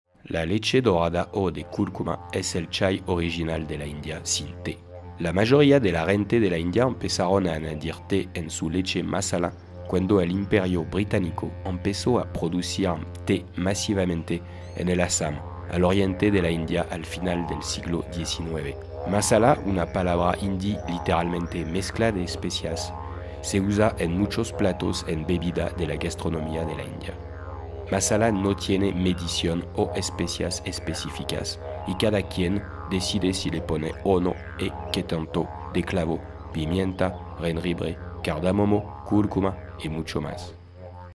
Breve historia del Masala Chai
La leche dorada o de cúrcuma es el chai original de la India sin té
la mayoría de la gente de la India empezaron a añadir té en su  leche masala
cuando el imperio británico empezó a producir té masivamente en el Assam, al oriente de la India, al final del siglo 19.
Masala, una palabra hindi, literalmente mezcla de especias, se usa en muchos platos y bebidas de la gastronomía de la India.
Masala no tiene medición o especias específicas y cada quien decide si le pone o no y que tanto de clavo, pimienta, jengibre , cardamomo, cúrcuma y mucho más.
Por esa razón se añadió el té al masala chai sin cambiar su esencia.